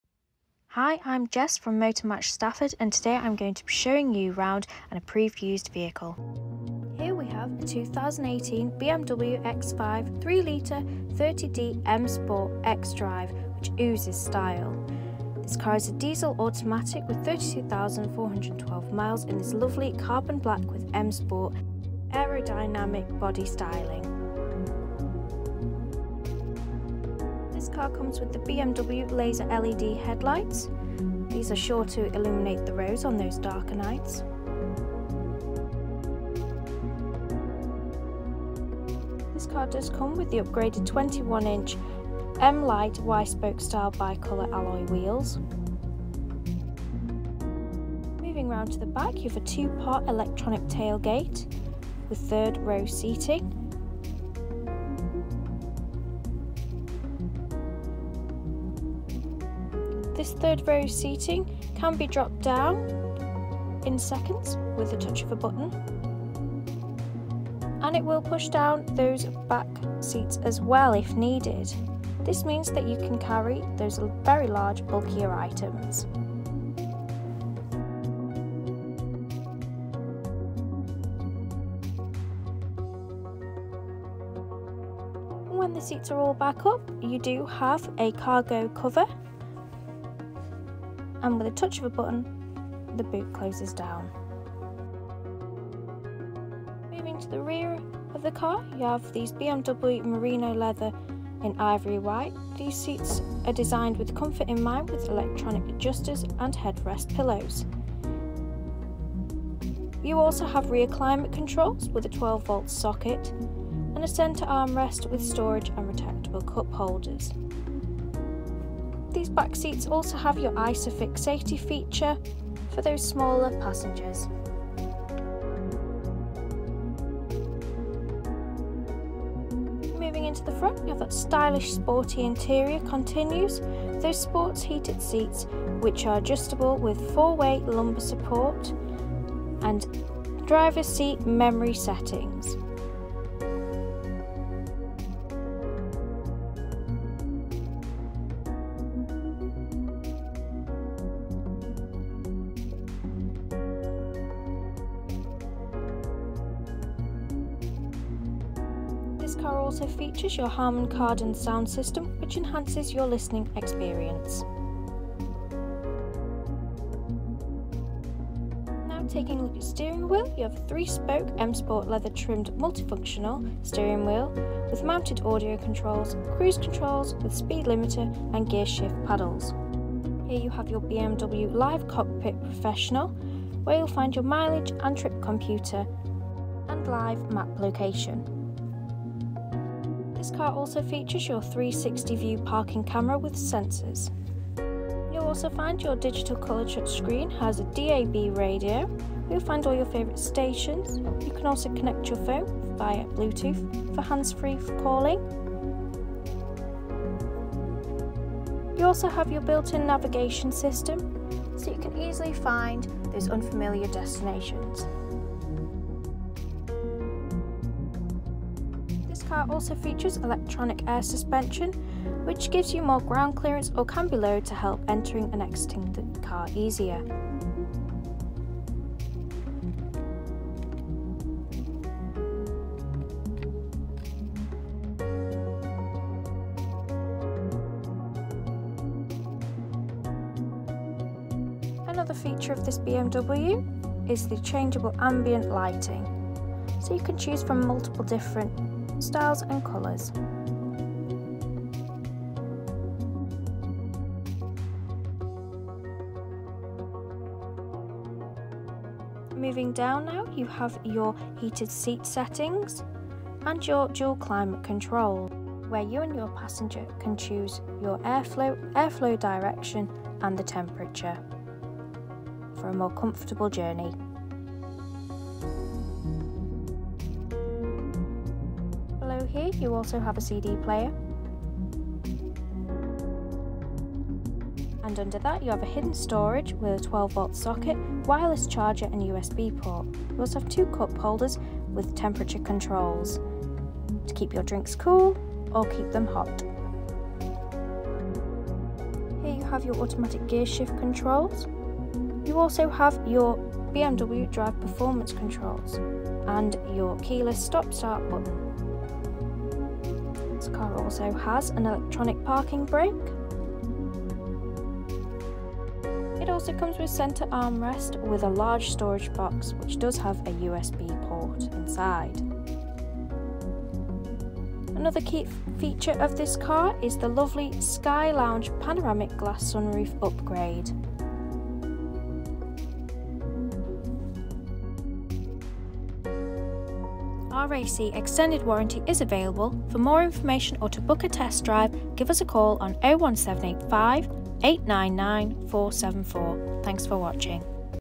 Used BMW X5 3.0 30d M Sport Auto x Drive | Motor Match Stafford
This stunning BMW X5 3.0 30d M Sport Auto x Drive has just come into stock at Motor Match Stafford.

This fabulous BMW X5 3.0 30d M Sport Auto x Drive is the perfect mixture of a high tech, reliable car which would be ideal for someone looking for a Luxury Spacious Car. The exterior of this car is beautiful and this can be seen in the LED Headlights which are good for illuminating the road ahead f you especially in those dark conditions. Another exterior feature which should be mentioned is the 21'' M Light Y Spoke Bi Colour Alloys, this compliments the Black exterior colouring, making the whole car stand out that much more. The interior is just as beautiful as the exterior and this can be seen in the Cream Leather Seats, this adds a unique aspect to the interior. These seats also provide the utmost comfort for not only the driver but for the passengers as well, this means that whoever has the pleasure of being in this car will have a memorable experience. The Harmon and Kardon Sound System can help enhance your listening experience on any journey you may take. The main focal point of the interior is the BMW Entertainment Display which has everything you may need for any journey you may take including; DAB Radio, Bluetooth Connectivity as well as a Navigation System to help you plan any journey you may take. Another high-tech Feature is the Head-Up Display which allows you to see the speed you are travelling at without taking your eyes of the road. This car also has a 7 Seat Configuration which allows you to carry 2 extra passengers.

Registration: YB68ZSV
Fuel: Diesel
Gearbox: Automatic

Key features include:
~ 7 Seat Configuration
~ LED Headlights
~ 360 View Parking Camera

OR